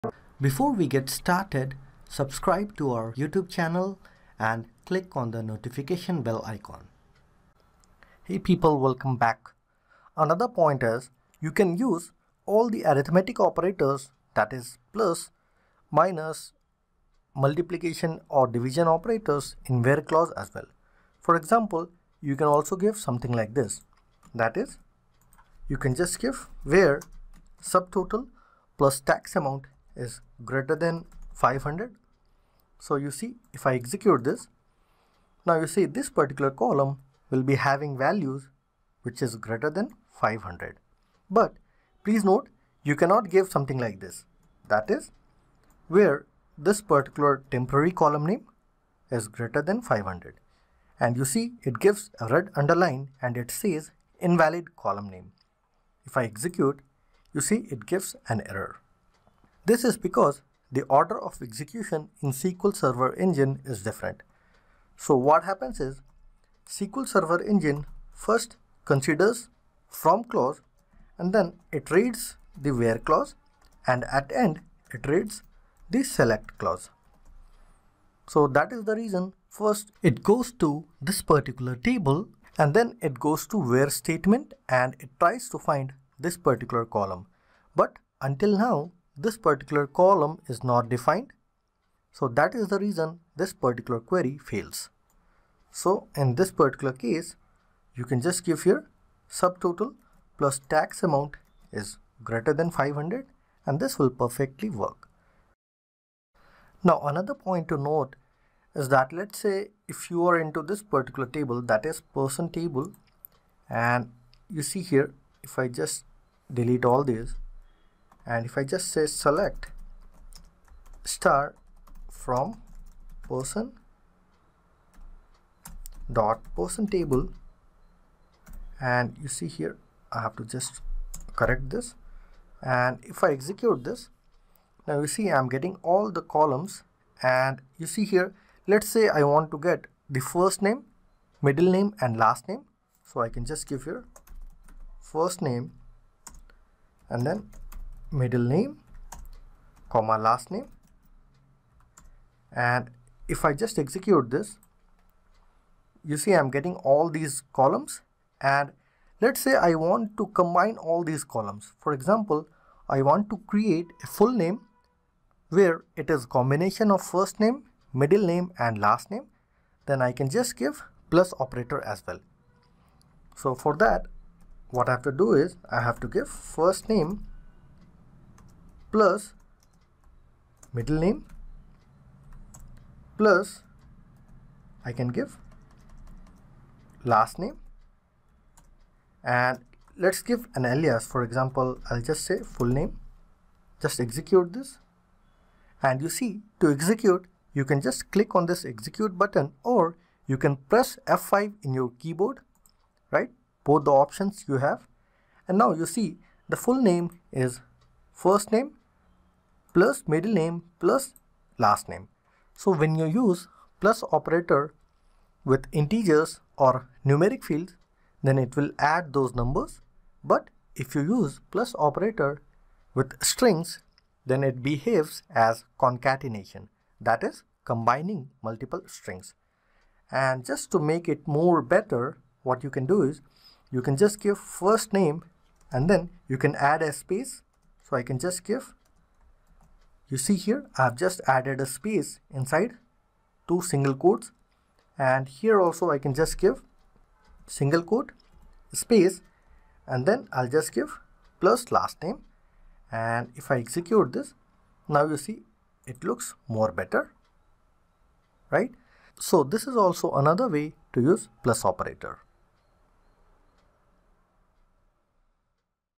How to Combine Strings in Microsoft SQL Server - Concatenation [2021]
This SQL Server tutorial shows you how to combine multiple strings using the Plus(+) operator in SQL. This is the part of the lesson of the crash course on Microsoft SQL Server